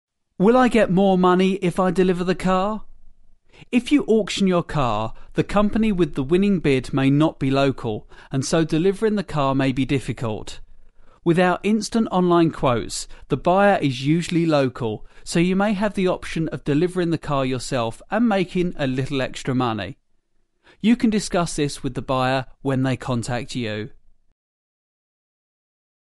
Will I Get more money if I deliver my Scrap Car?
If you auction your car, the company with the winning bid may not be local, and so delivering the car may be difficult. With our instant online quotes, the buyer is usually local, so you may have the option of delivering the car yourself and making a little extra money. You can discuss this with the buyer when they contact you.